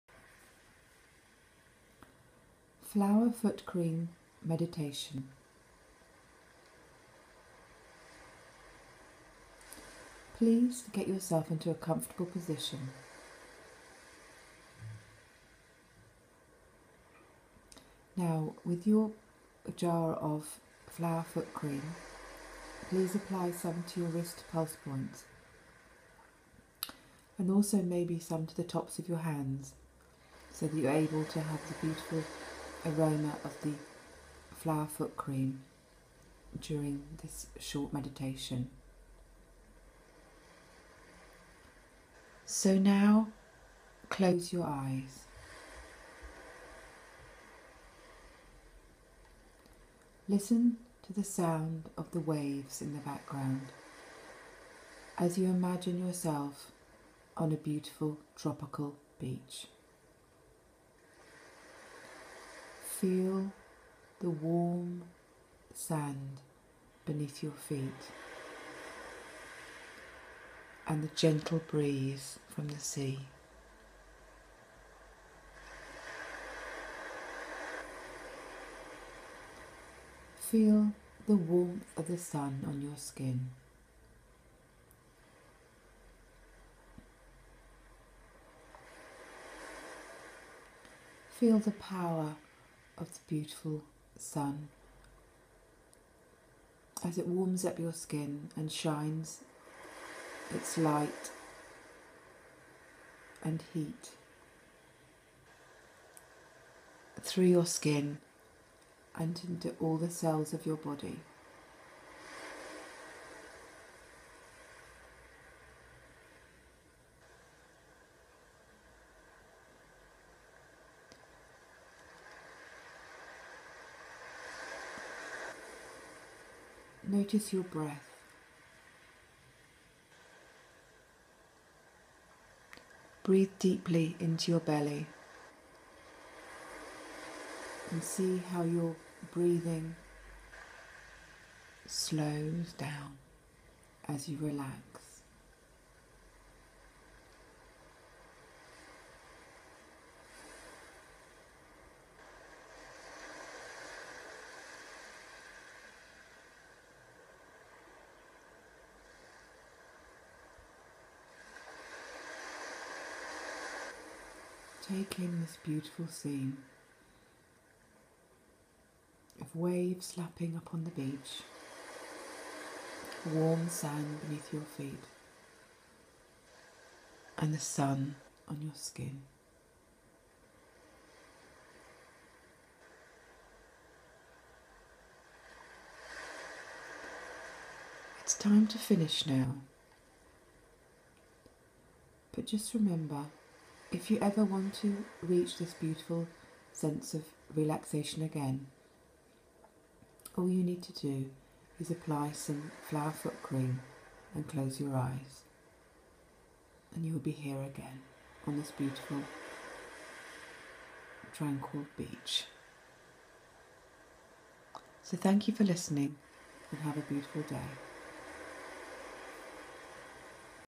Flower Foot Cream Short Meditation
Apply some Flower Foot Cream for this short guided meditation. Imagine yourself on the tropical beach where 2 of the essences for this cream were created.